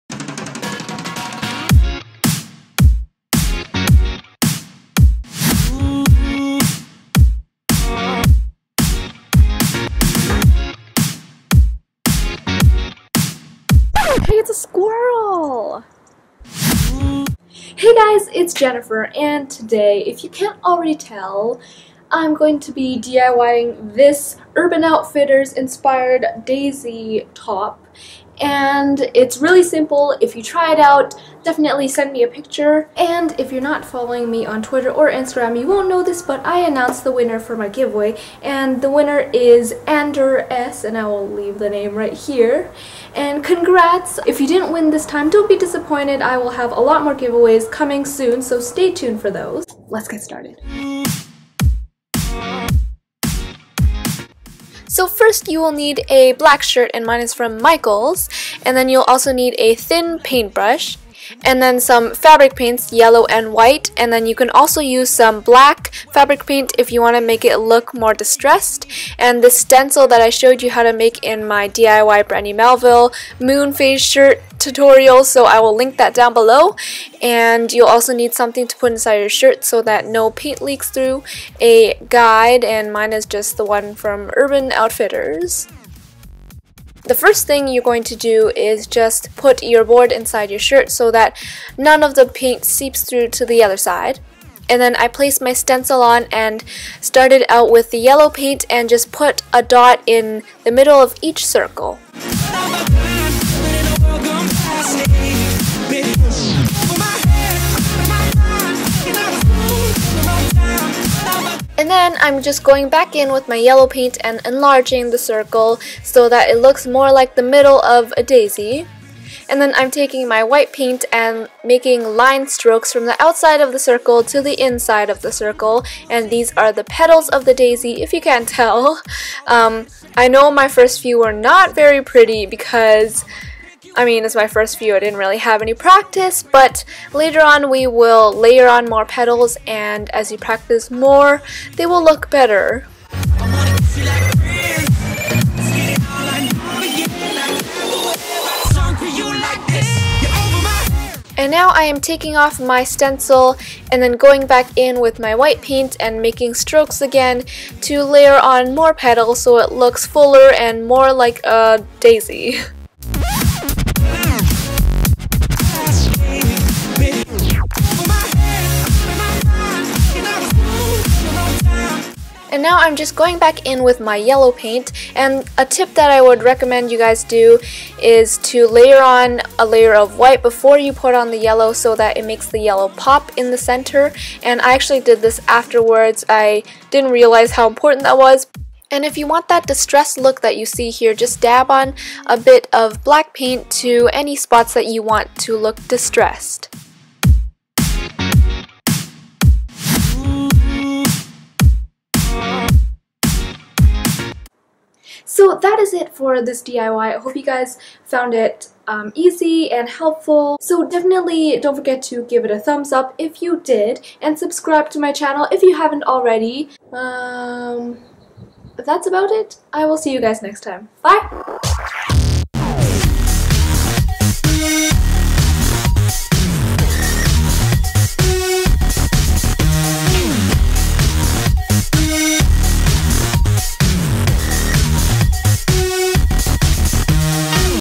DIY: Daisy Graphic shirt | tumblr and urban outfitters inspired daisy crop top
Hey guys!

Materials:
-plain black shirt (Michaels)
-thin paintbrush
-white, yellow, and optional black fabric paint (Michaels)
-board
-a picture reference